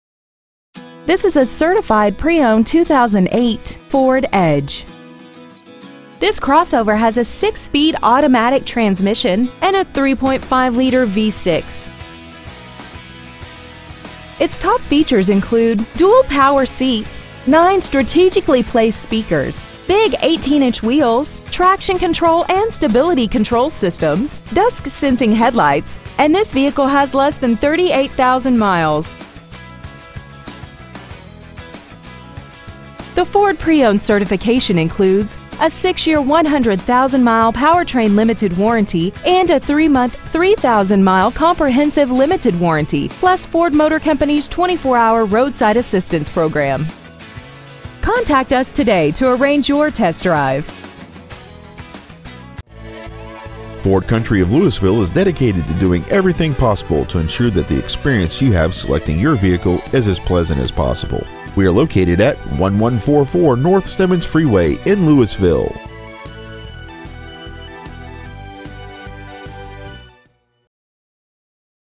2008 FORD EDGE Certified TX
Year : 2008
 Make : FORD
 Model : EDGE
 Engine : 3.5L V6
 Trans . : AUTO 6SPD
 Exterior : GRAY
 Miles : 37,965
 Interior : CAMEL
 Stock : BBA53012A